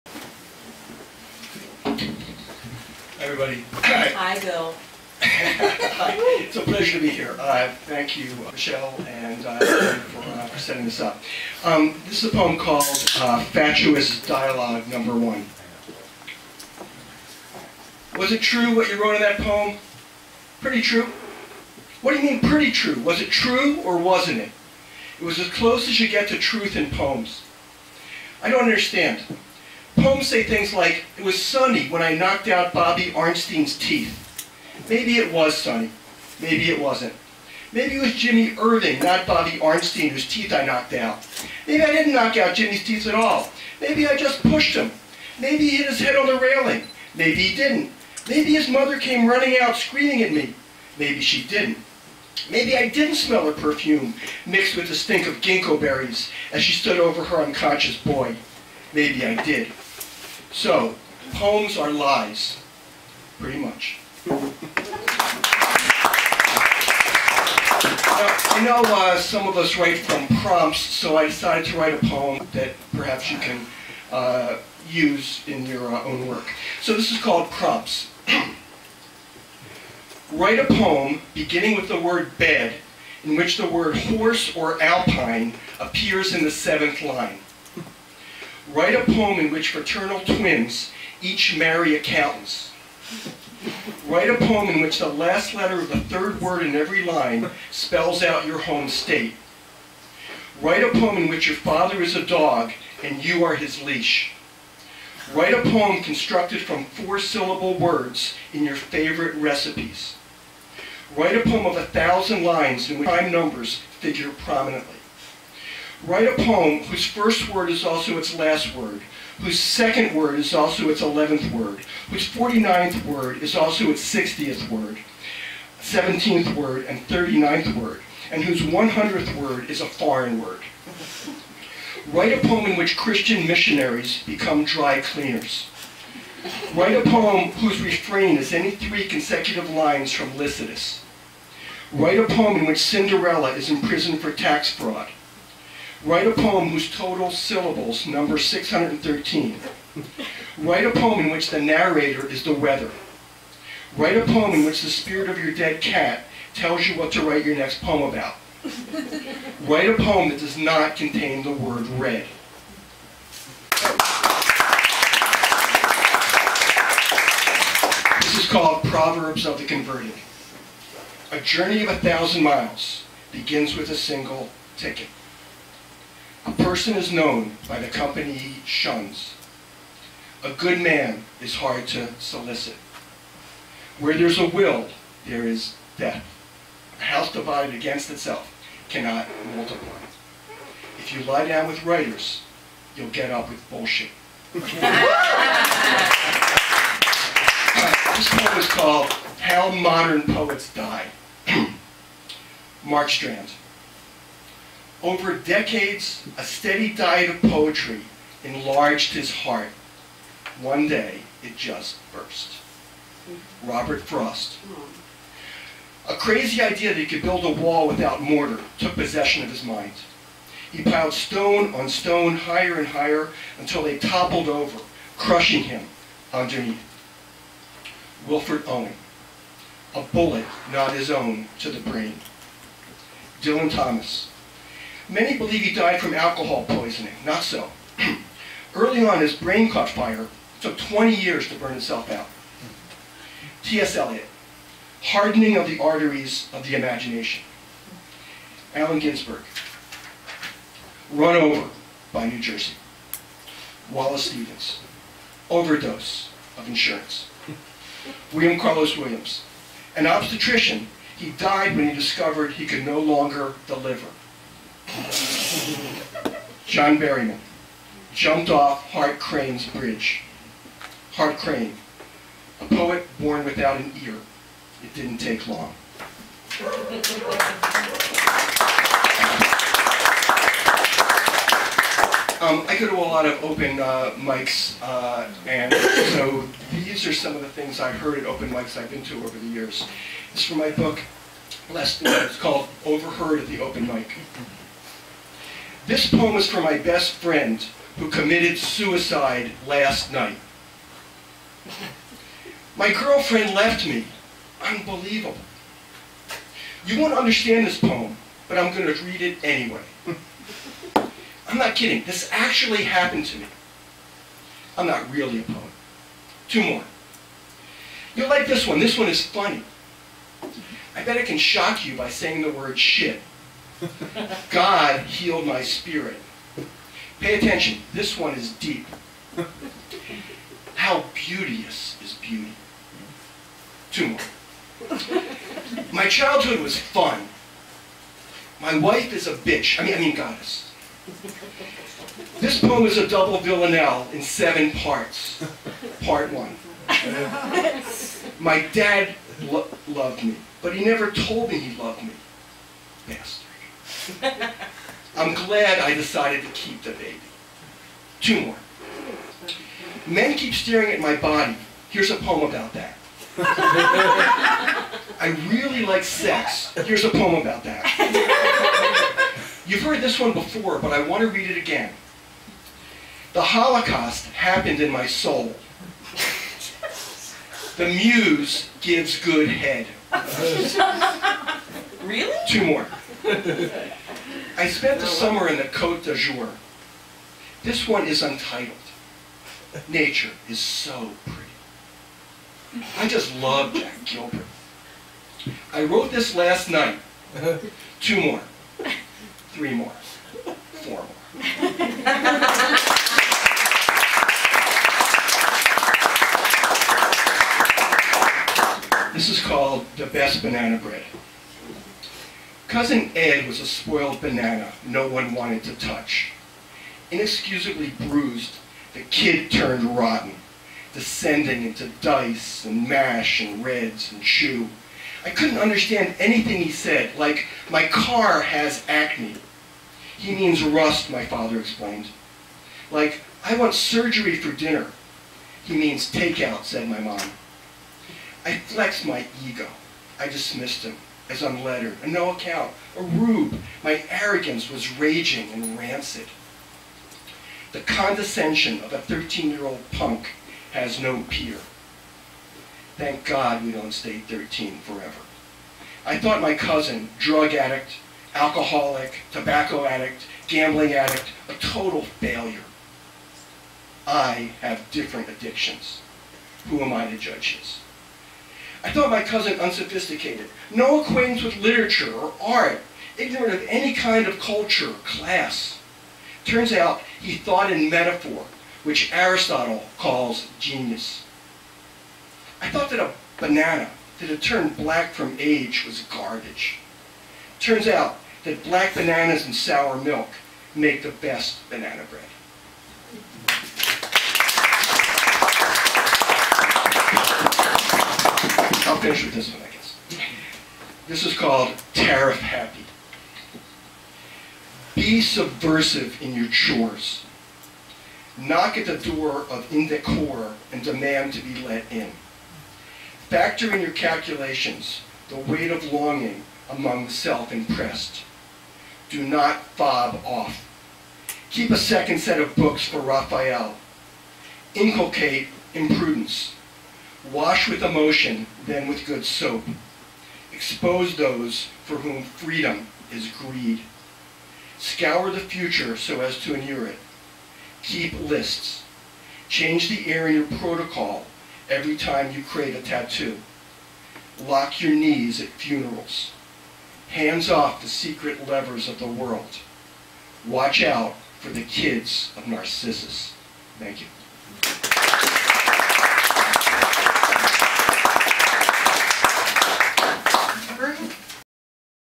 Bill Yarrow reading at at The Blue Heron in La Porte, Indiana, on November 7, 2015
Bill Yarrow reading at The Poetry Bash at The Blue Heron in La Porte, Indianaon November 7, 2015. Release party for This is Poetry: Volume II: The Midwest Poets, edited by Michele MacDannold.